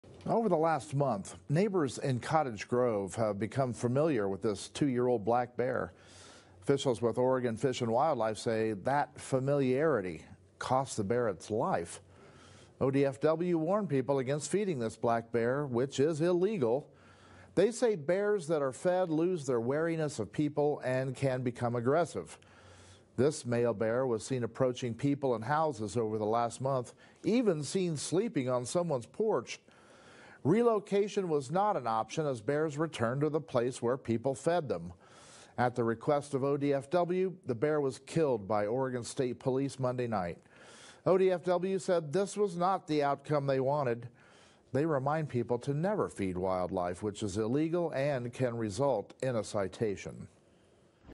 Black bear fed by people in Cottage Grove put down to protect humans
A 2-year-old black bear in Cottage Grove that was intentionally fed by people had to be put down by officials this week after it lost its wariness and began approaching people, ODFW says.